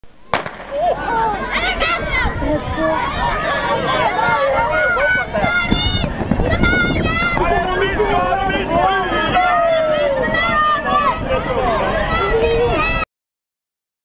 Cedric Goffin op Averegtenjogging te Heist-op-den-Berg in 2007 - deel 1